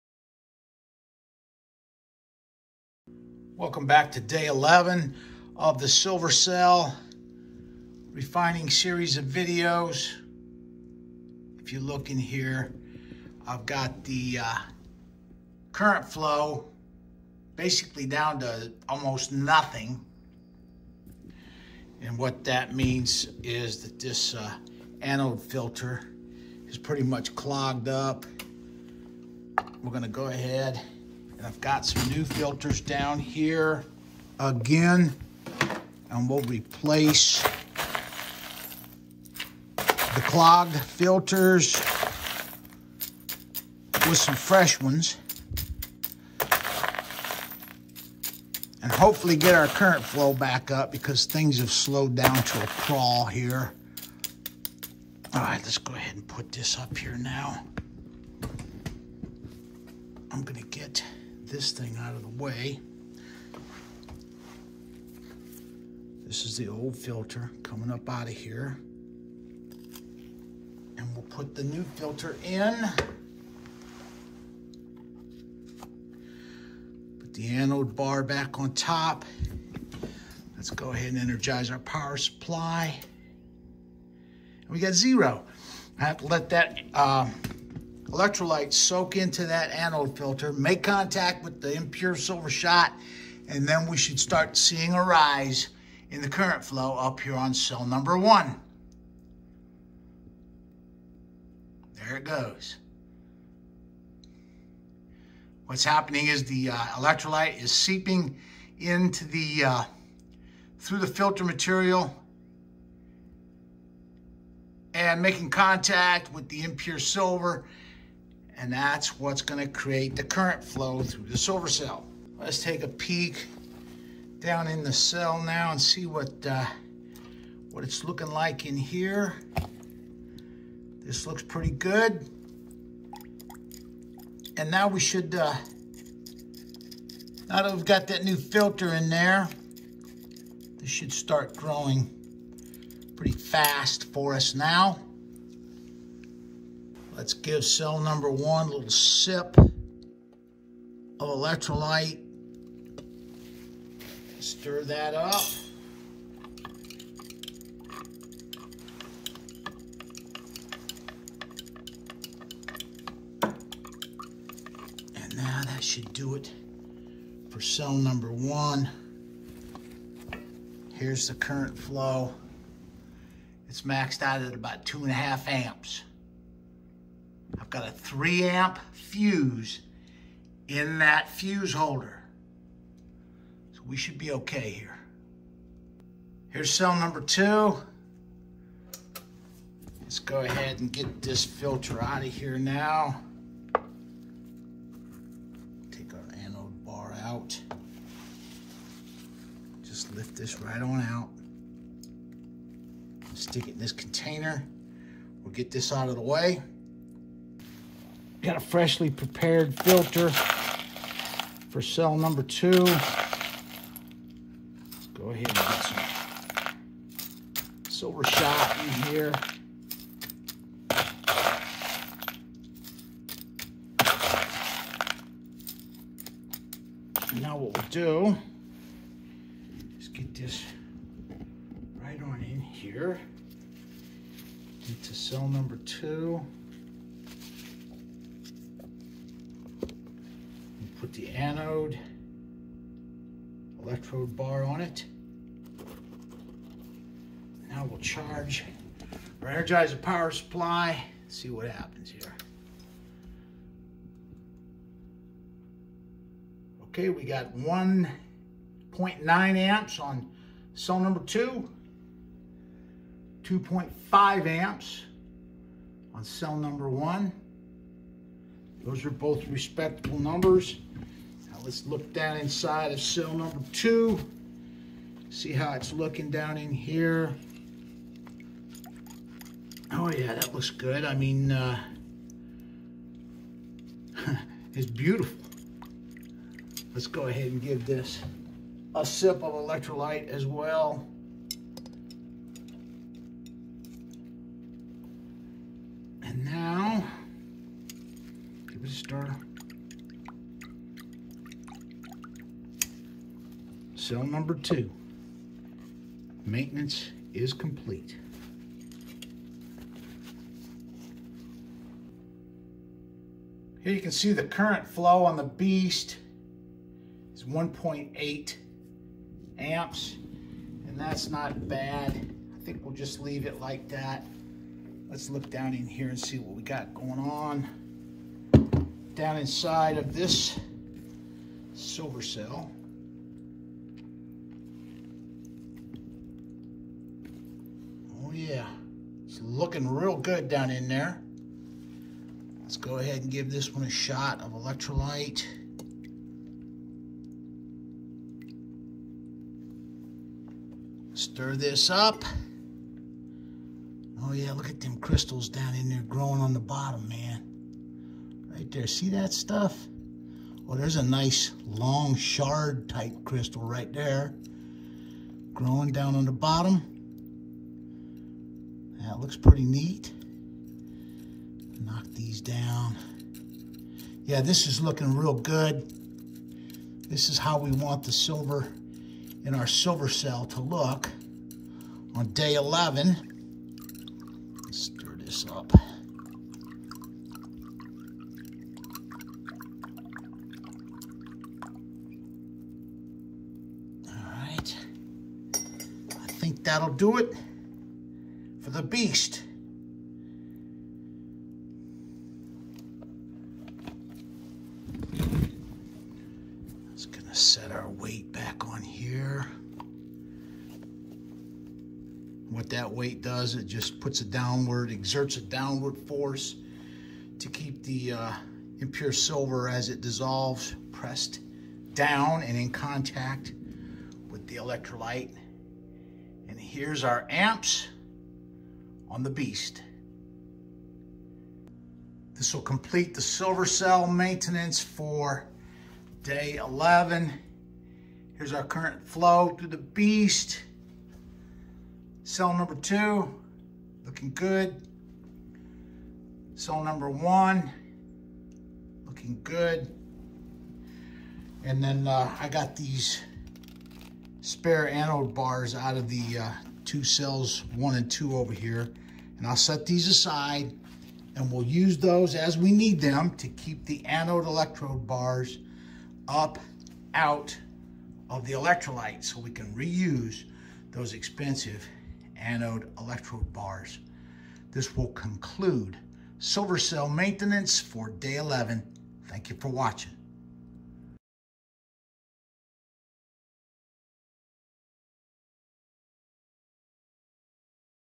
Silver Cell Day 11 Crystal Growth 264 Hours
Eight Day Time Lapse Pure Silver Crystal Growth Music